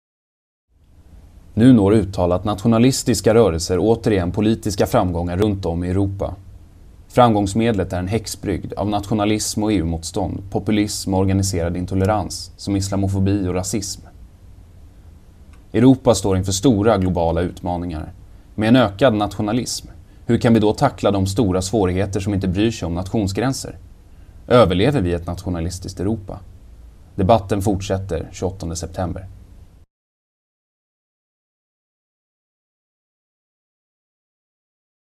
Nationalismens Europa
Den uttalade nationalismen har gjort politisk comeback över hela Europa, i kölvattnet av eurokrisen och populistiska partiers framgångar. Därför bjuder Global Utmaning och Expo in till ett halvdagsseminarium om Nationalismens Europa den 28 september i Stockholm. För mer information och anmälan se

Creative Commons photo credits, Flickr: Leigh Phillips, Thomas Gerard, Aliwinst, Tilj Vercaemer, Orazio Esposito, Noborder network, Sara Prestianni.

Speaker: Erik Glaad.